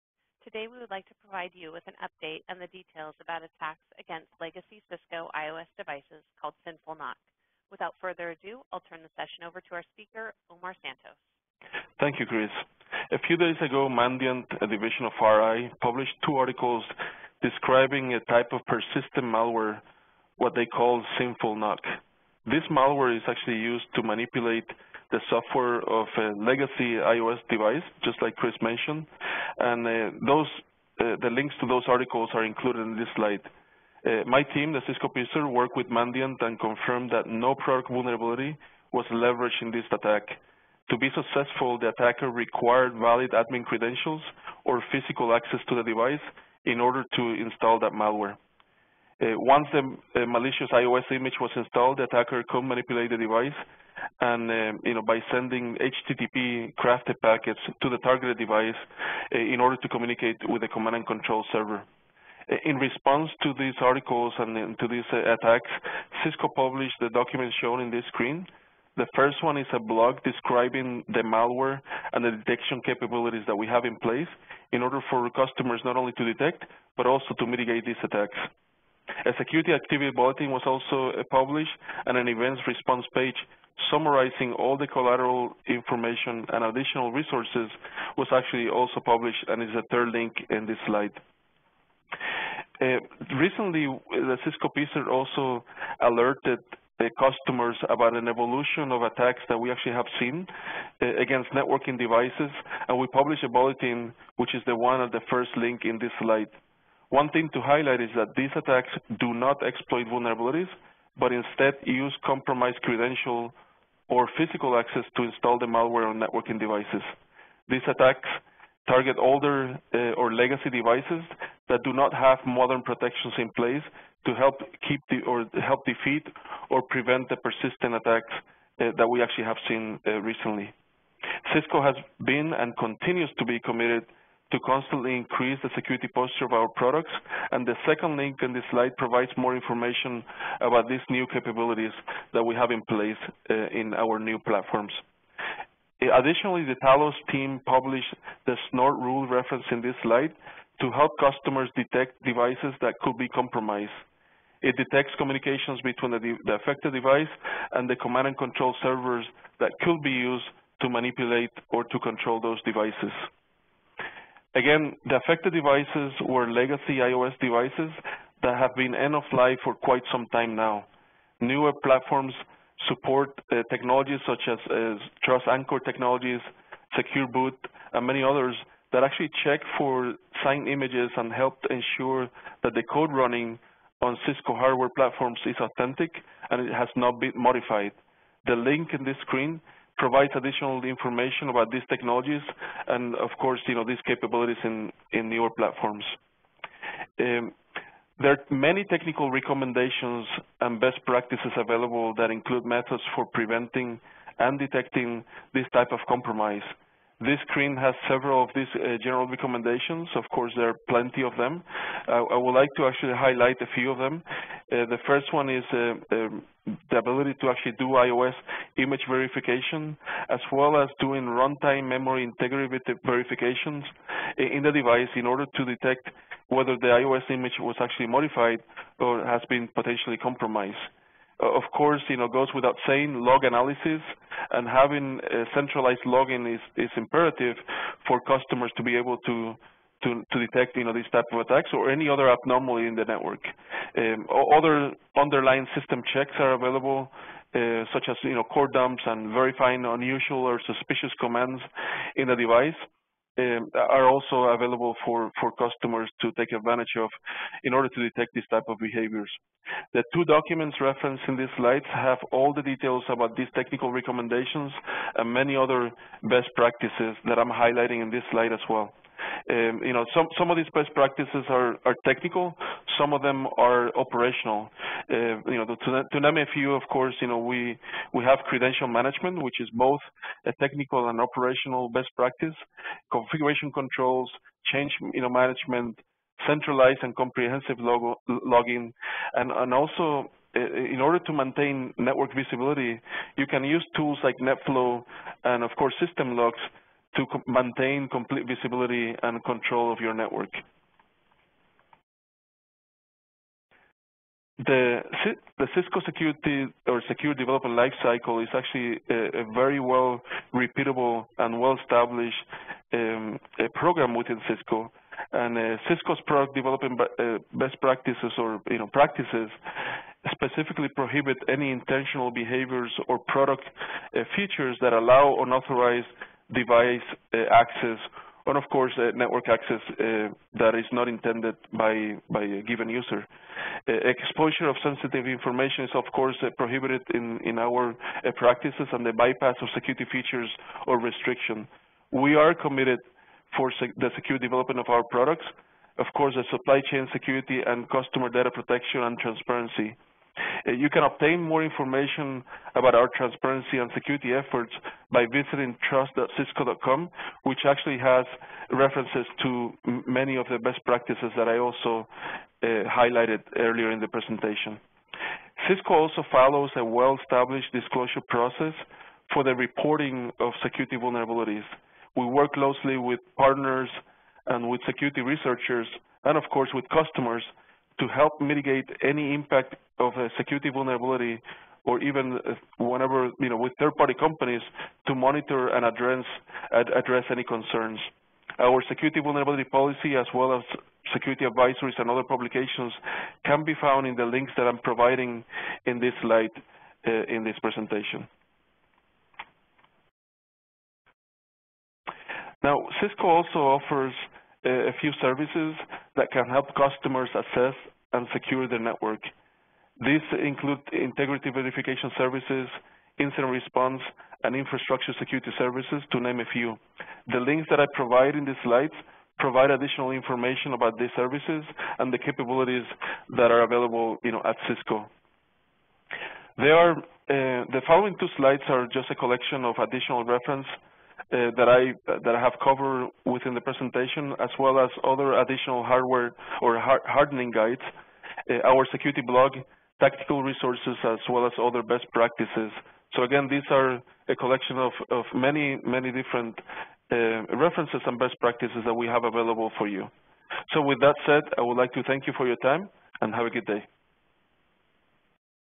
SYNful Knock: Soluții pentru detectarea și remedierea atacurilor
Pe 15 septembrie, Cisco și Mandiant (FireEye) au publicat informații despre un nou tip de virus, numit "SYNful Knock”. În clipul acesta, Omar Santos,  Cisco Principal Engineer, descrie cum acționează SYNful Knock și oferă detalii care îi pot ajuta pe clienți să detecteze și să remedieze viitoare atacuri.